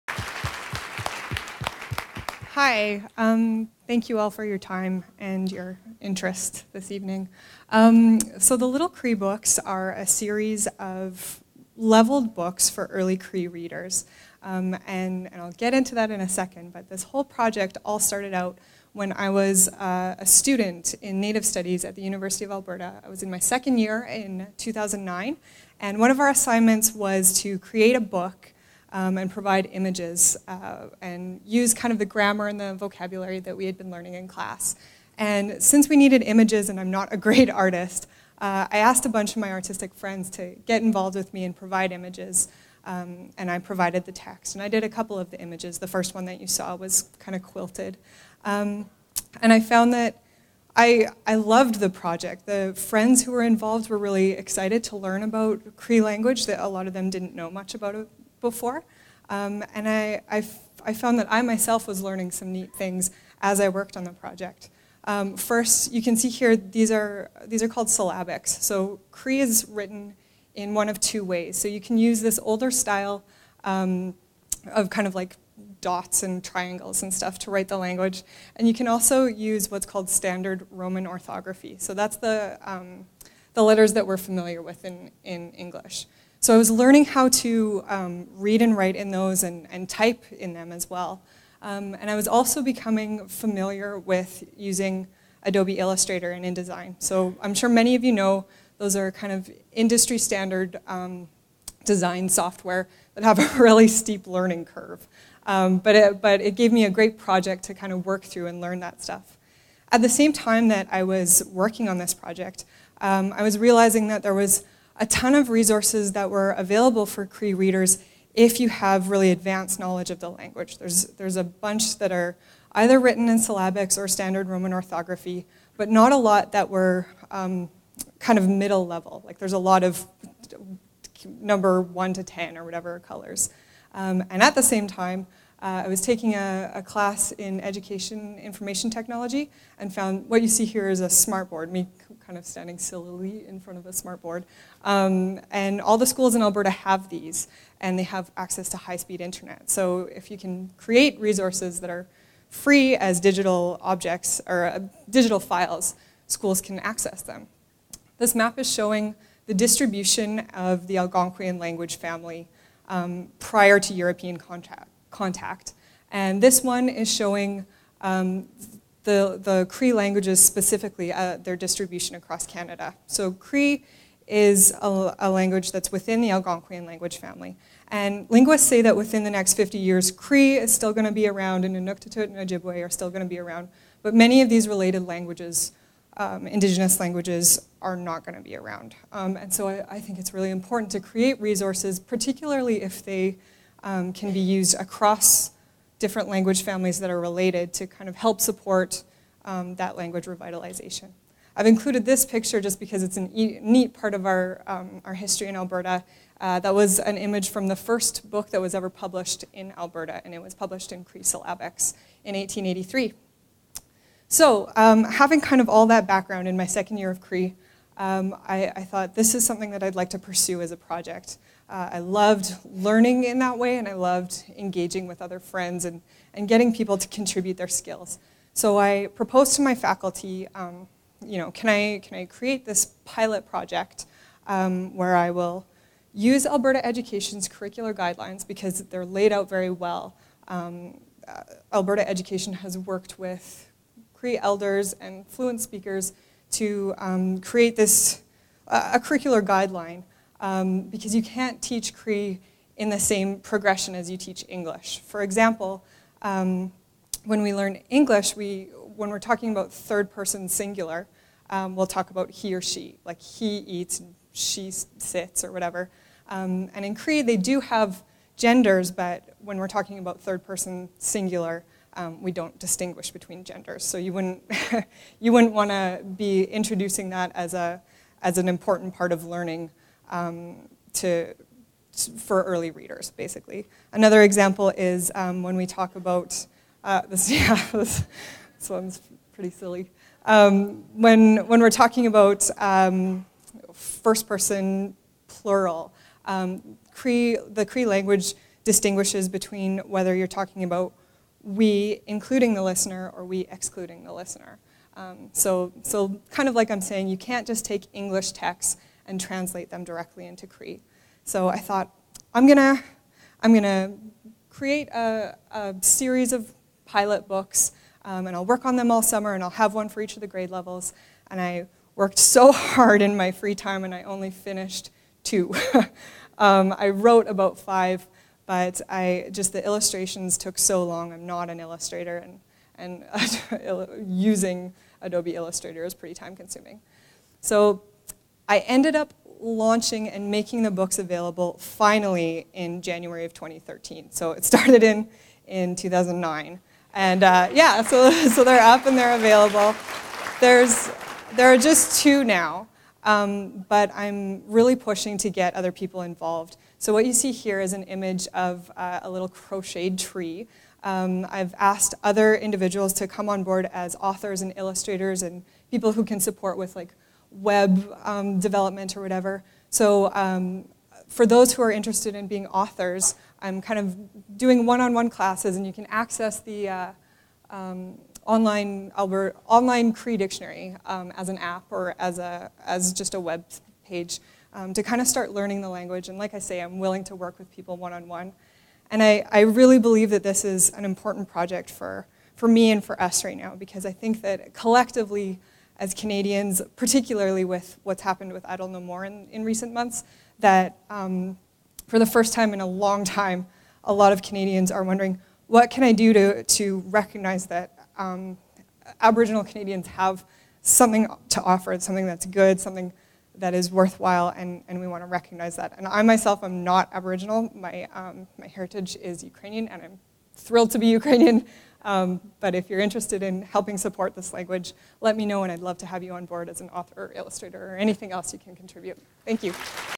Pecha Kucha 15 - Caylie Gnyra - Little Cree Books
Caylie Gnyra with Language Ally talks about Little Cree Books at the 15th Pecha Kucha night on March 7, 2013.